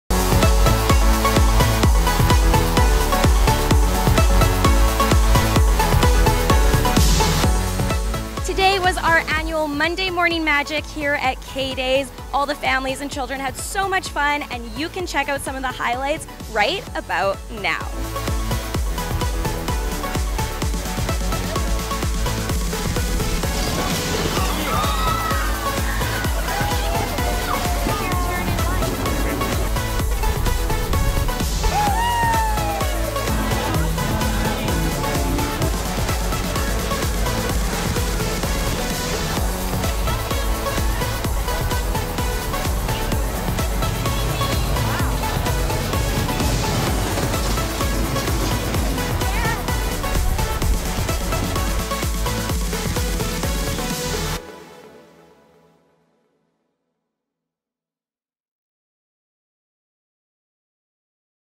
Northlands TV - Monday Morning Magic
Our NTV crew caught some of the sights and sounds of Monday Morning Magic, perhaps the most special event that occurs at KDAYS each year.